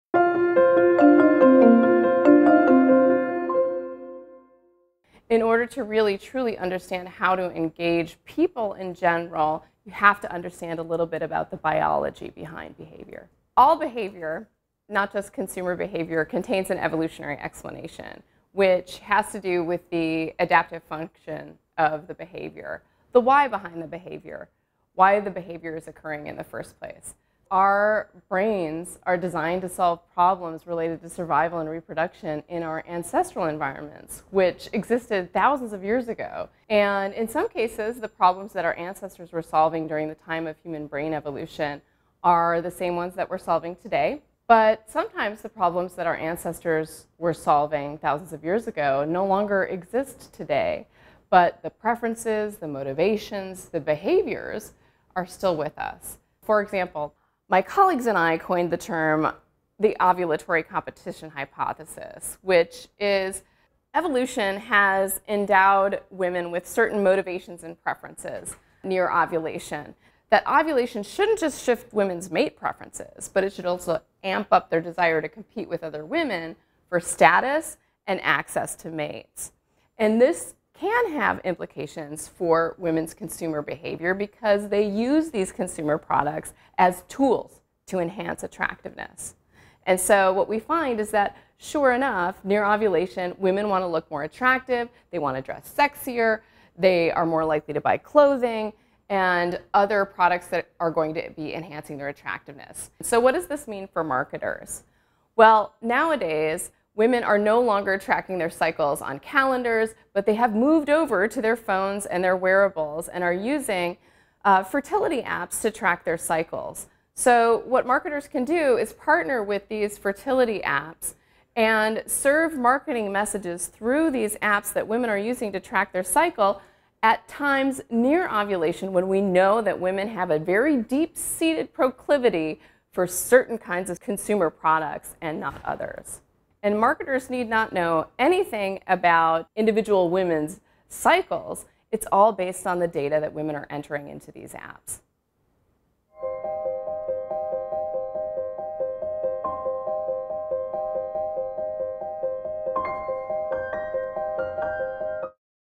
Rutgers Business Insight: Marketers use knowledge of evolutionary behavior to sell products
| Rutgers Business School Marketing associate professor and marketing PhD program coordinator Kristina Durante discusses how evolution influences consumer behavior, the Ovulatory Competition Hypothesis, and how marketers are leveraging human biology to sell goods.